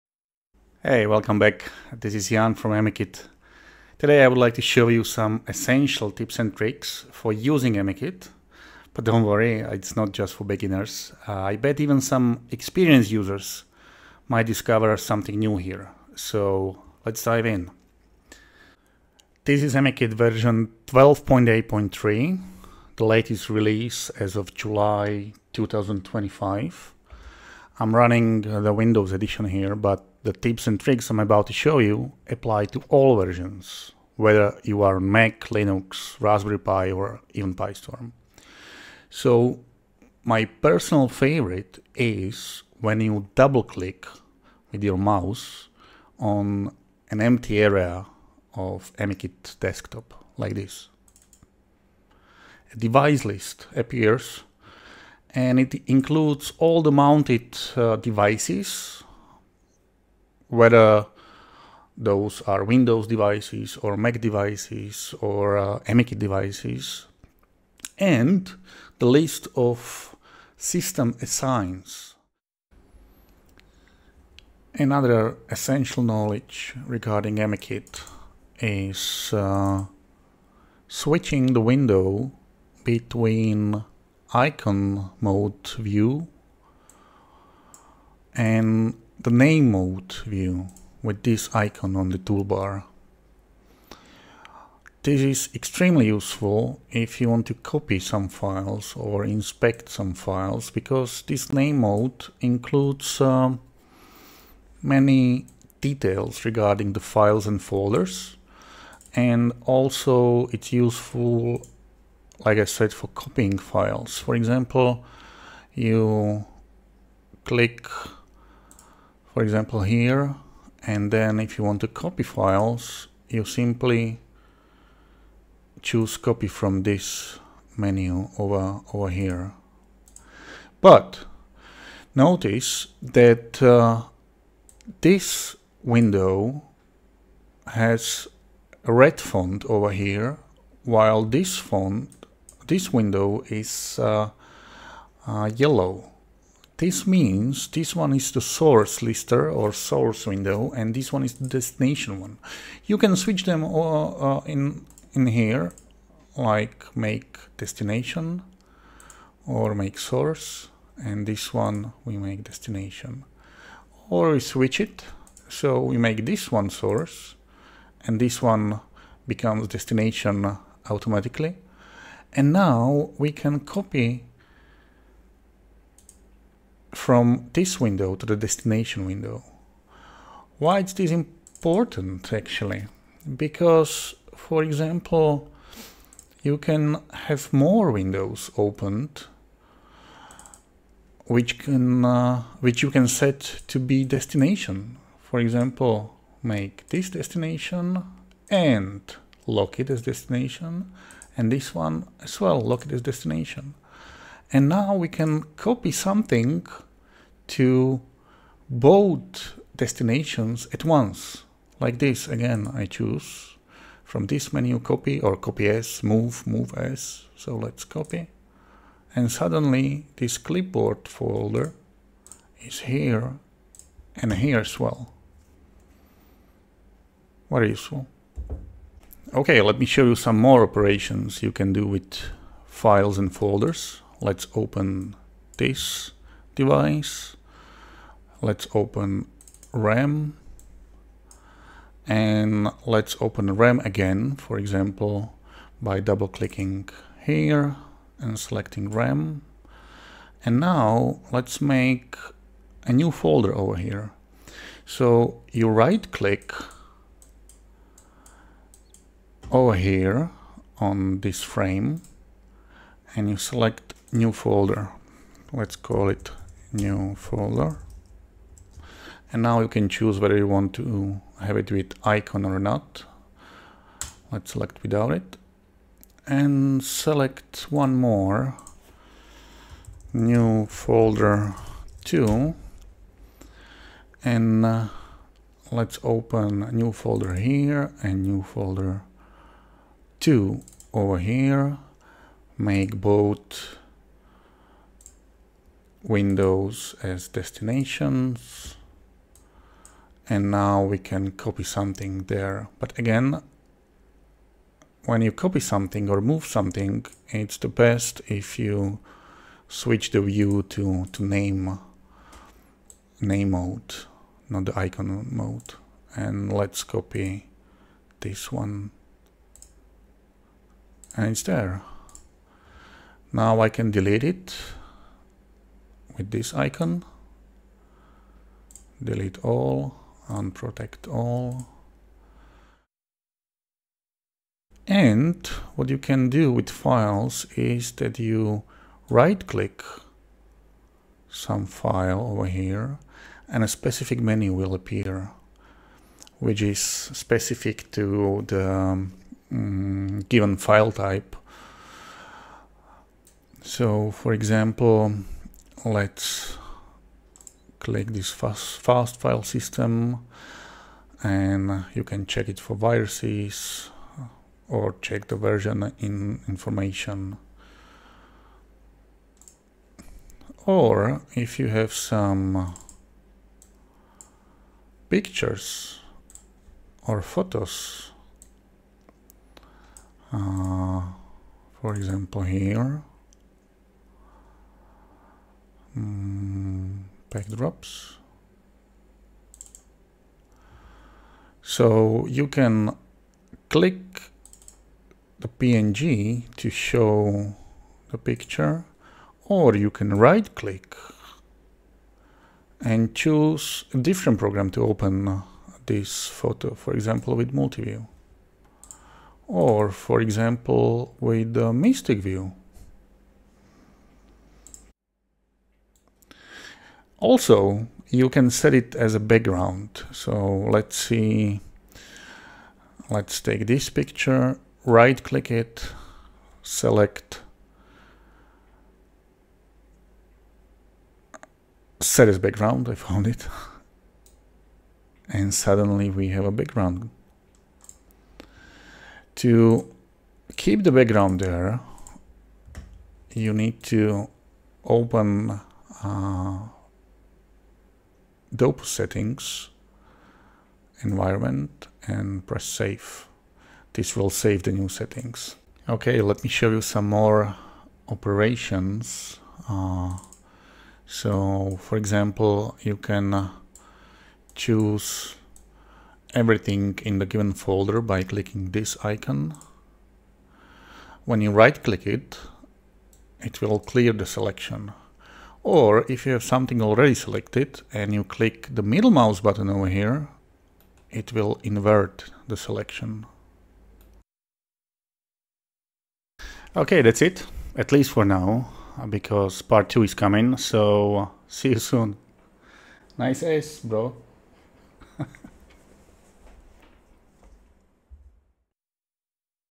AmiKit Essentials - Part 1
00:00 Intro
00:39 Double-click on Desktop
01:20 Icon vs Name View
01:52 Copying files
03:06 Multiple Destinations 1
04:10 Make New Folder
04:43 Multiple Destinations 2
05:23 Delete Files
05:39 Right-Click Files
06:57 Change Wallpaper
07:42 Select/Invert Files
08:05 Outro